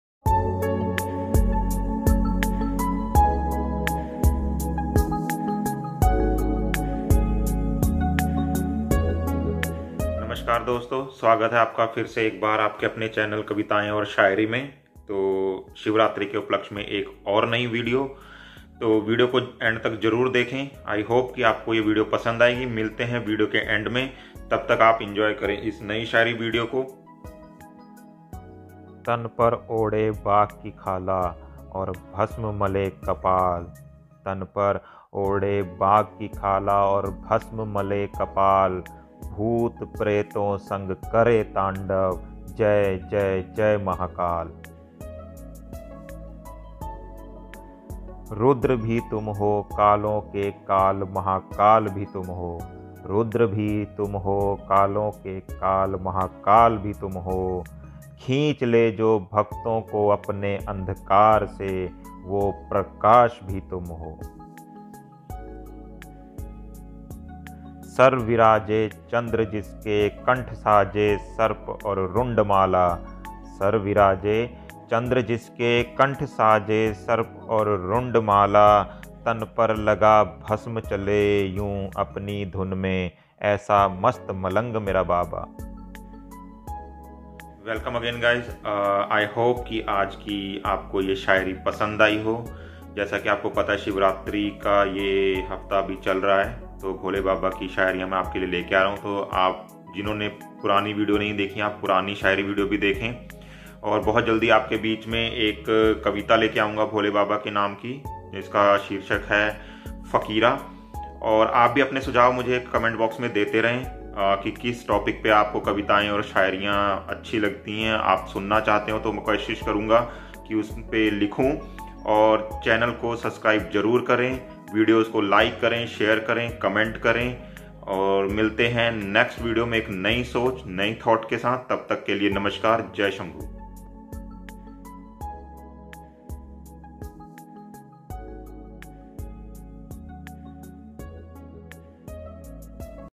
Mahashivratri special har har mahadev | hindi shayari | do you know Lord Shiva different names ?
Do you know about Shiv? Do you know about Bholenaath? Do you know about Neelkanth? Here is all information about Lord Shiva . In this video you will see Why shiv know as Bhasmdhaari, Neelkanth and Other Names. How Shiva get called bholenaath.